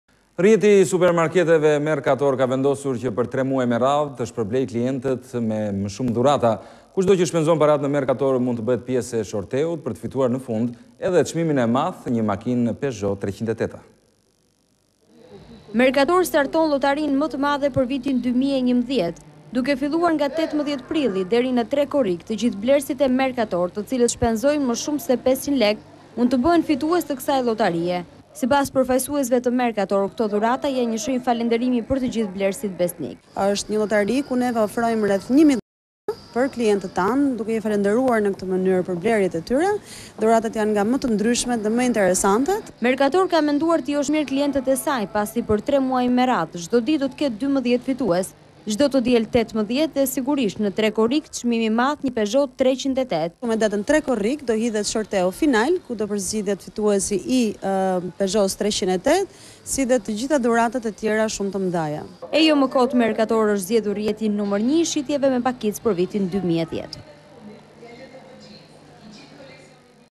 Vizion Plus - Lotaria ne Mercator
Lotaria më e madhe e Mercator nga data 18 Prill deri në 3 Korrik 2011!
Çdo blerje prej 500 Lekë ju jep shansin të fitoni Peugeot 308 & 1.000 dhurata të tjera!!!
Makina larëse dhe Frigoriferë Gorenje, pajisje kuzhine të ndryshme, kupona prej 10.000 Lekë dhe 2.000 Lek, dhe shumë e shumë të tjera!
Çdo ditë 12 fitues!
Mercator. Shijoni blerjet!

Big winning game from Mercator Albania from 18th April until 3rd July 2011!
Every purchase of at least 500 Leke in any Mercator store, gives you the opportunity to win a Peugeot 308 or 1.000 other prizes!!!
Washing machines and refrigerators Gorenje, vouchers of 10.000 Lek and 2.000 Lek, different kitchenware and many many more!
Every day 12 winners!
Mercator, Enjoy shopping.